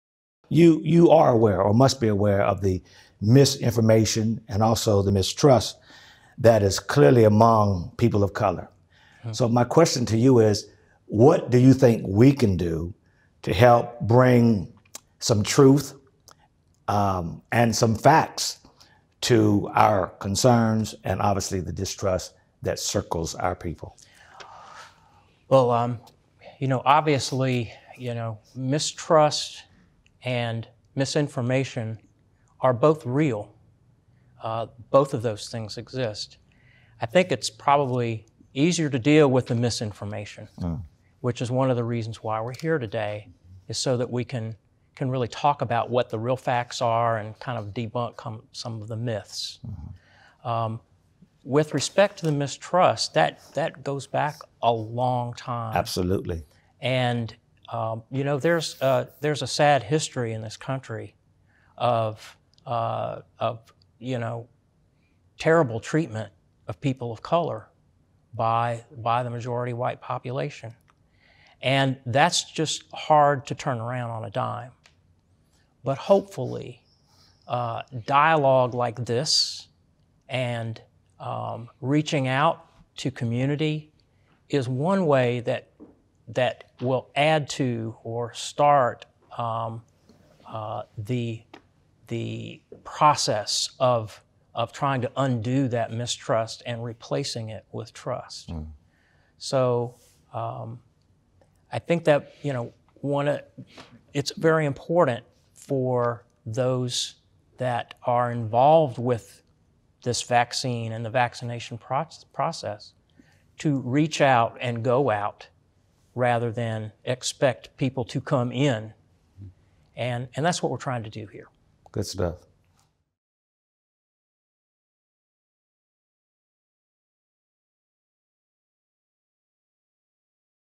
COVID-19 Vaccines Conversation: Addressing Mistrust and Misinformation
Union County Medical Director Dr. Stephen Keener sits down with two well known local pastors, Bishop Osco E. Gardin, Jr. and Pastor Melvin Tate, to answer questions about the COVID-19 vaccines, particularly questions from historically marginalized populations.